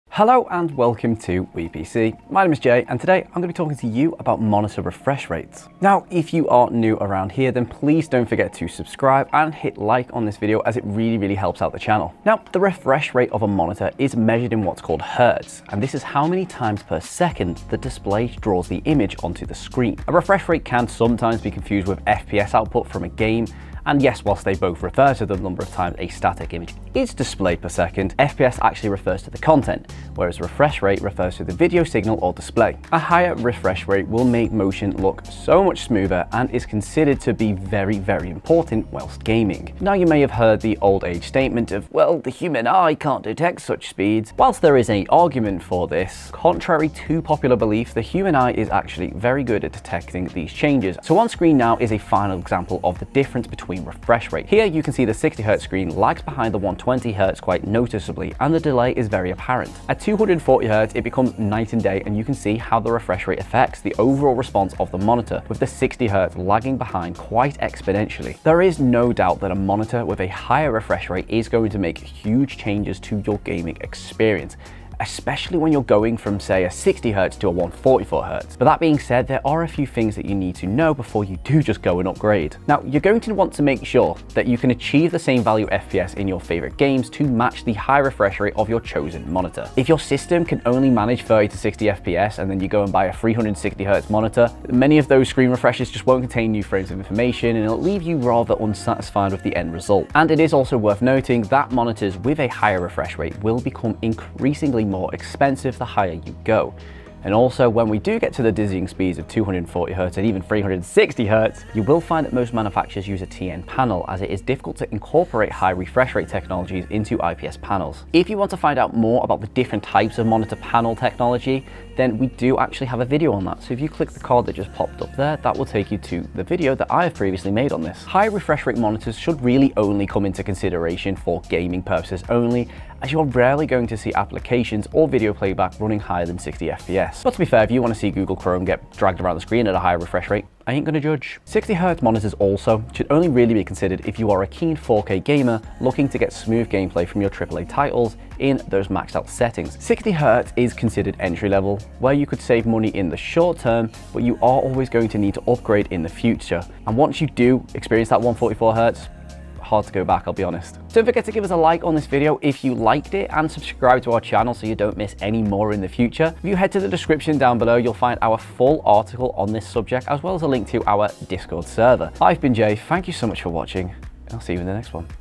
Monitor Refresh Rates Explained - Does it Make a Difference?
In this video Jay will talk about monitor refresh rates and the differences between them. As well as talking about the benefits of potentially upgrading to those higher refresh rates!